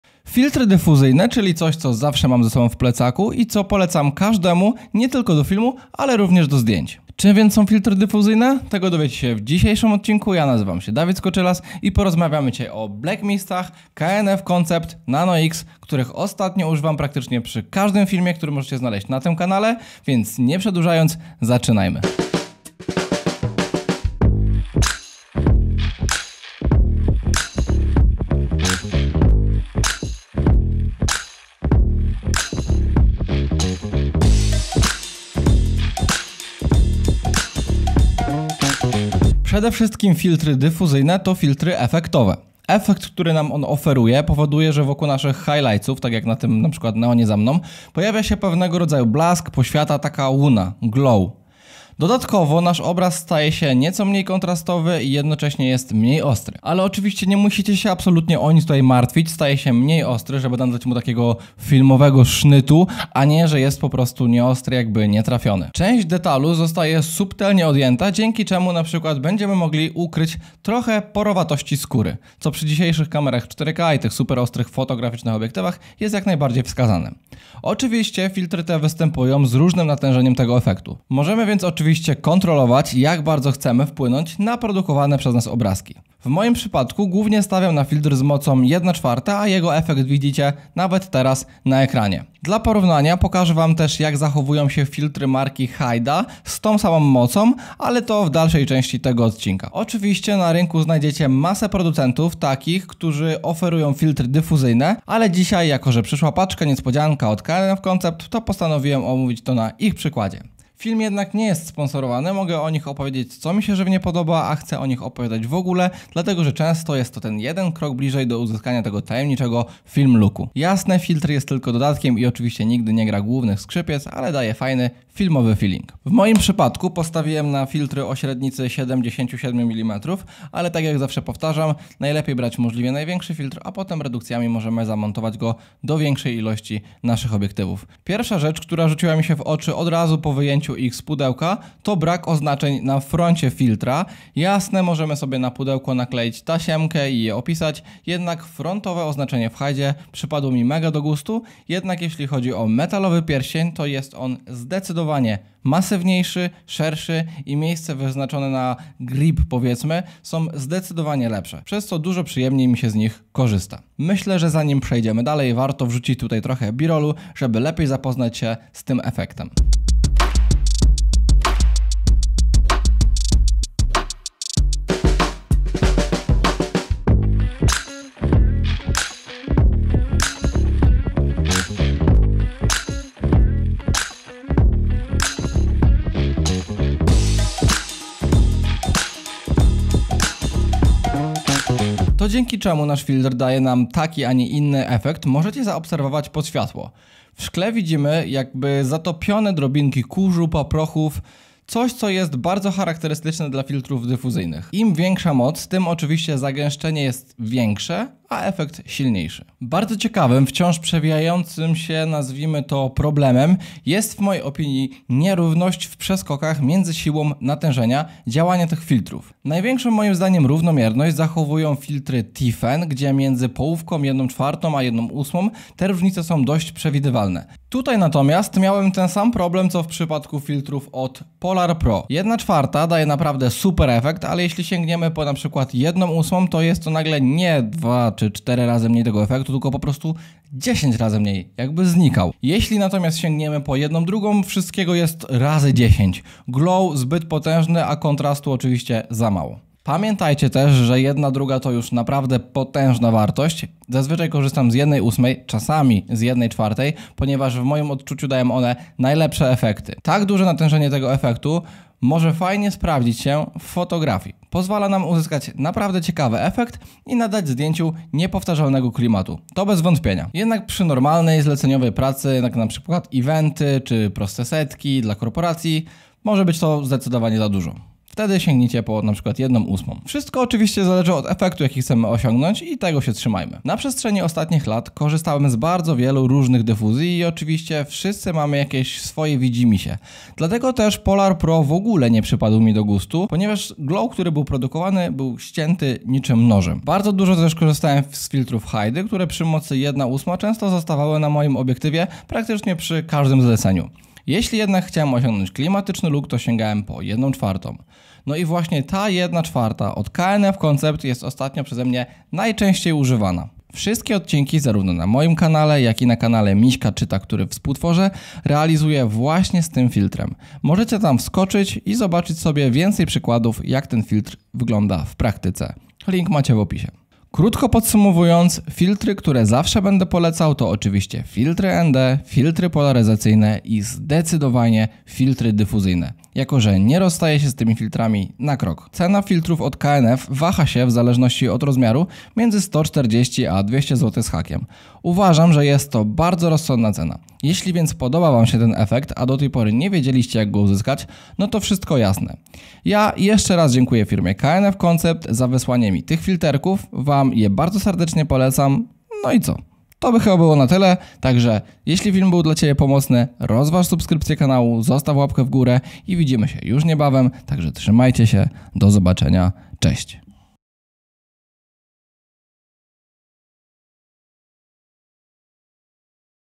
Filtry Dyfuzyjne - Czym są i jak działają? - K&F Black Mist
Jeśli zastanawiałeś się jak w filmie lub zdjęciach inni osiągają efekt poświaty, blasku, pewnego rodzaju glow, to jest to film dla Ciebie. Filtry dyfuzyjne często nazywane ProMist przez wzgląd na najbardziej popularny model, to filtry które dają właśnie wyżej wymieniony efekt. Często ludzie psorzypisują mu tak zwany Film Look.

W tym odcinku opowiadam o tych filtrach na podstawie K&F Concept Black Mist Nano-X. Jeśli jest to efekt który Cię zainteresował, to z czystym sumieniem polecić ten produkt. Link znajdziecie poniżej.